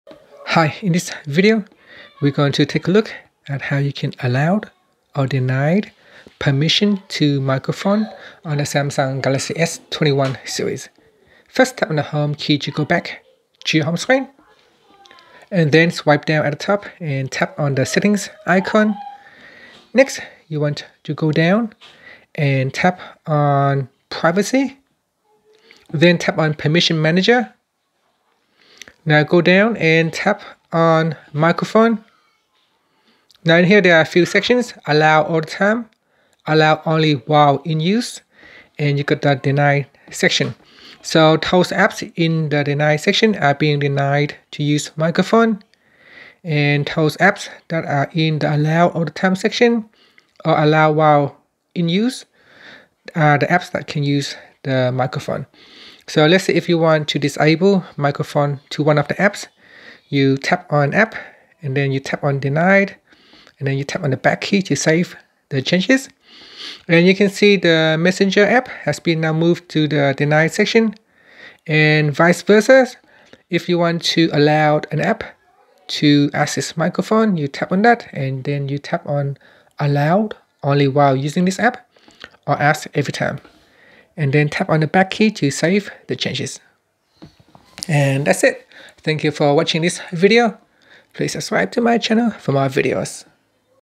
Galaxy S21/Ultra/Plus: How to Allow/Deny Permission to Microphone
Learn how you can allow or deny permission to the microphone on the Galaxy S21/Ultra/Plus.